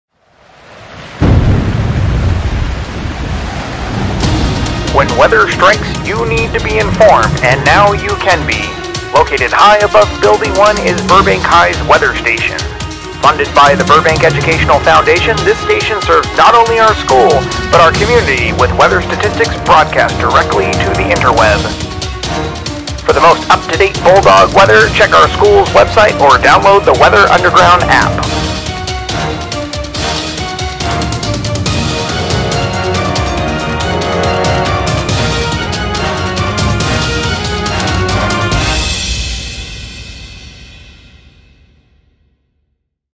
Burbank High School Weather Station
This video is about the Burbank High Weather Station funded by the Burbank Educational Foundation.